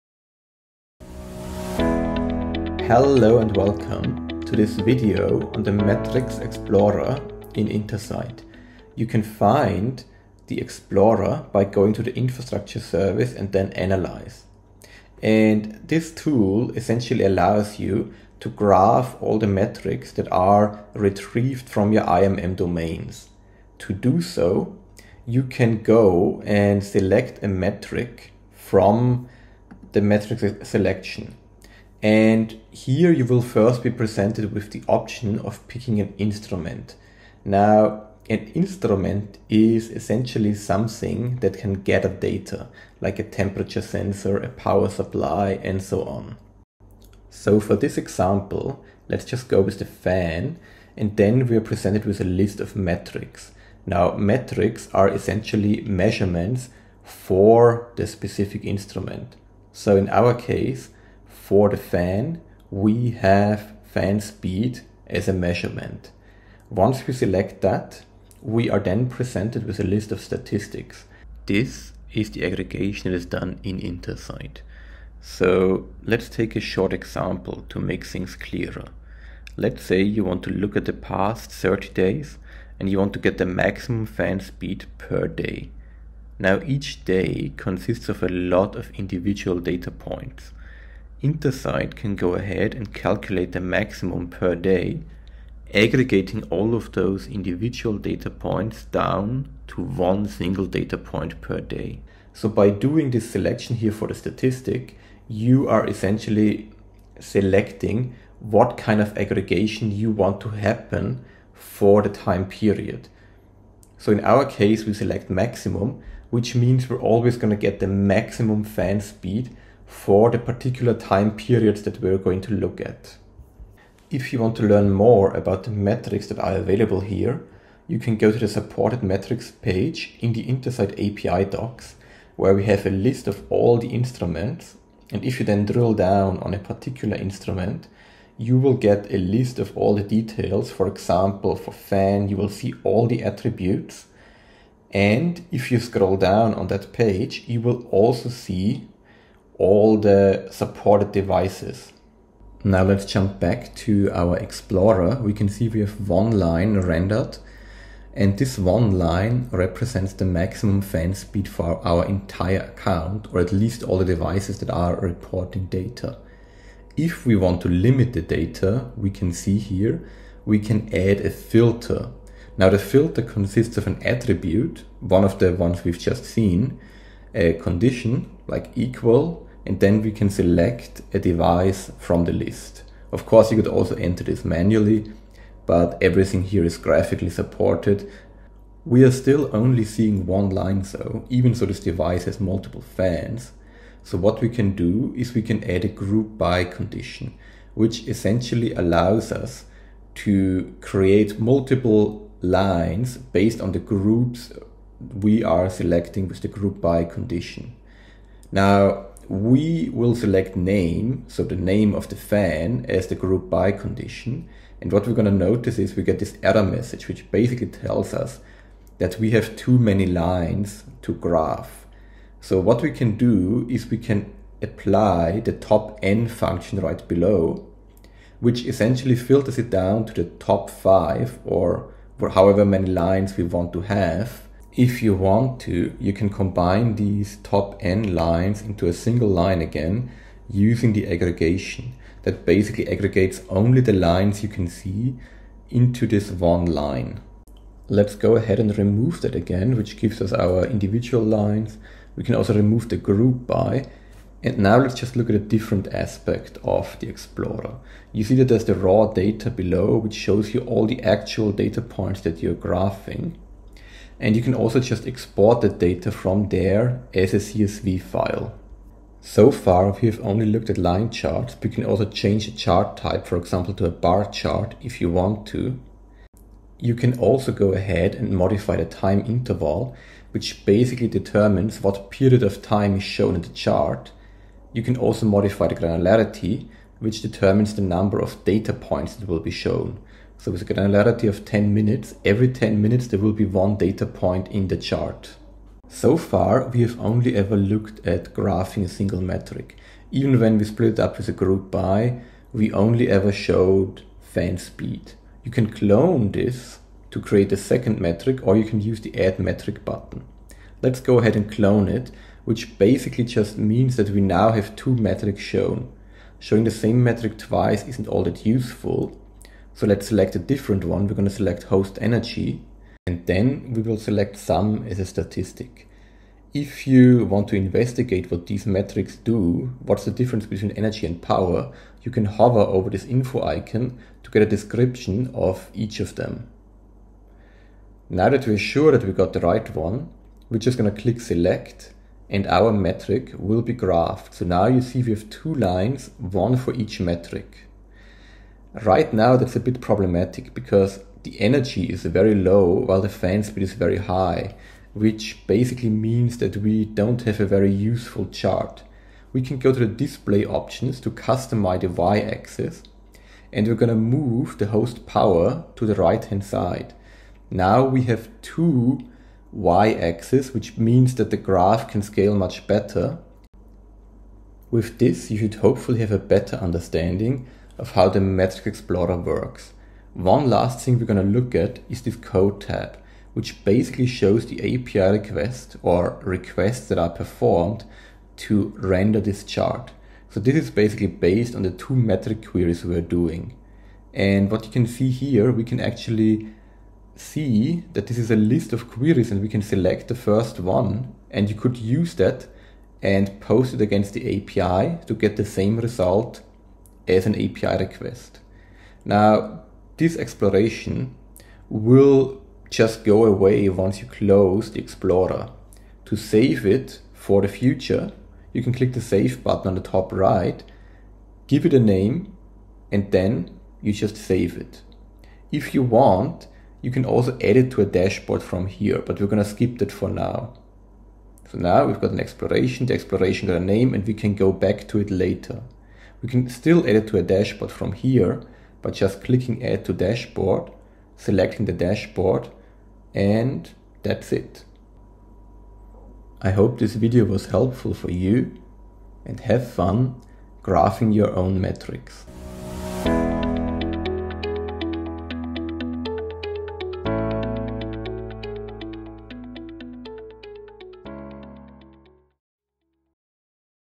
Understanding the Metrics Explorer
Instructional video on how to create metrics explorations in Cisco Intersight.

The metrics reference mentioned in the video can be found